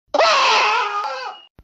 Brandon the Quaquaval scream
By Beating The tandemaus in final round on new york city